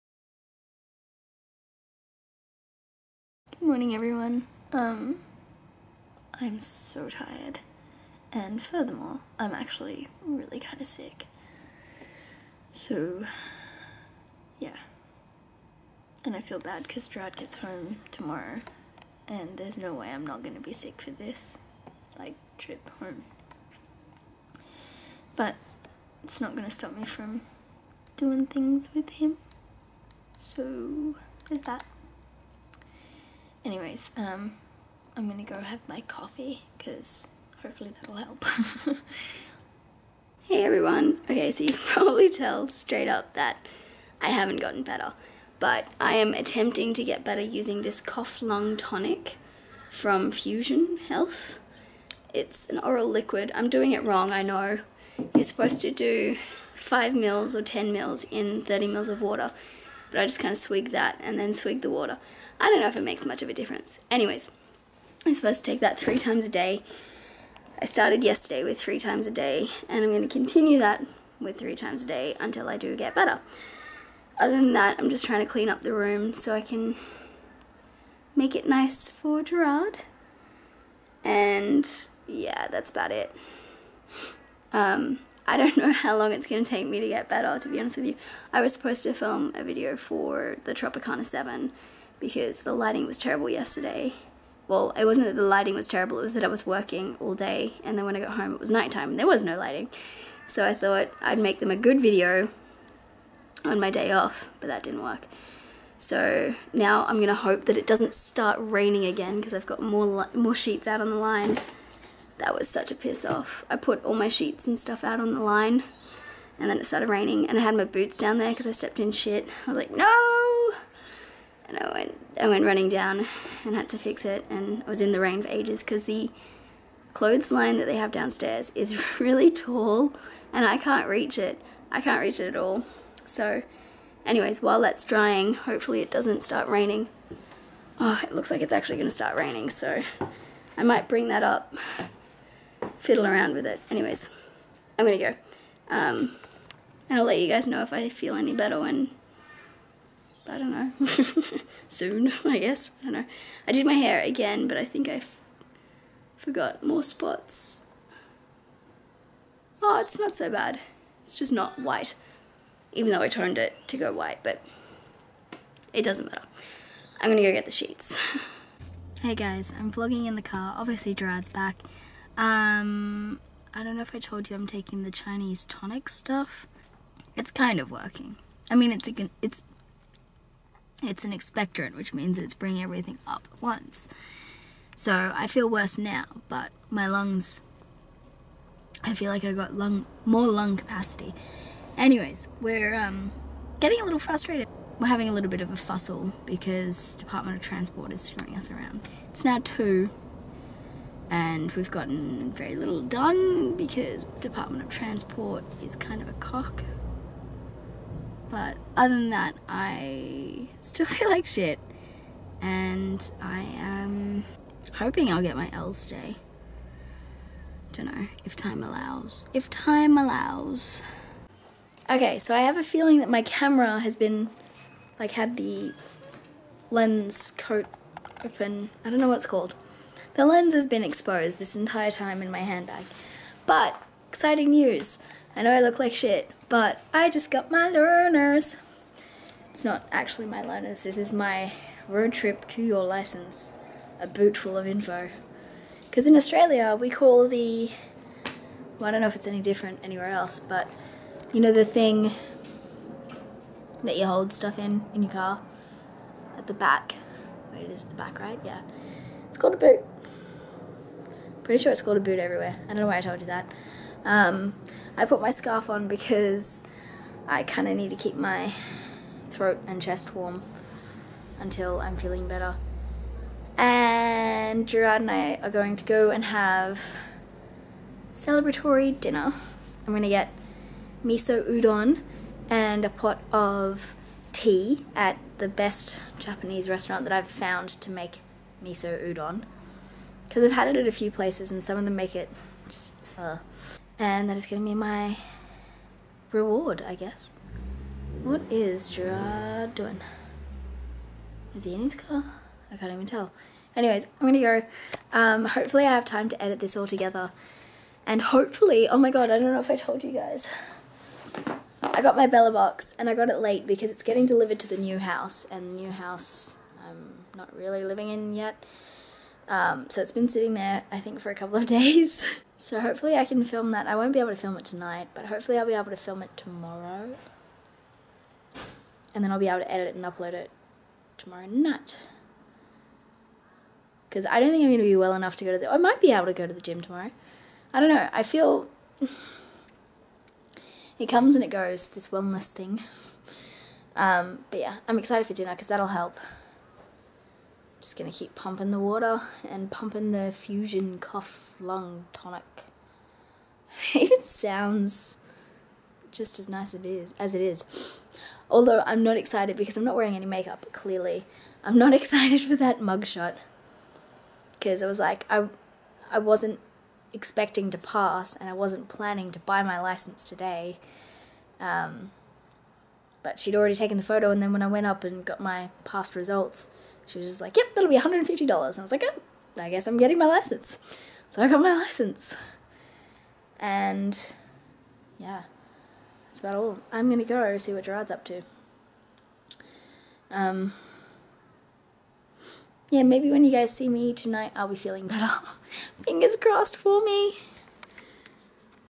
SICK AS A DOG! 23/07/14 MsAverageKate Vlogs
And the saddest part is I am now officially back to work after my 2 days off, feeling no better at all. Wish me luck serving customers and everything!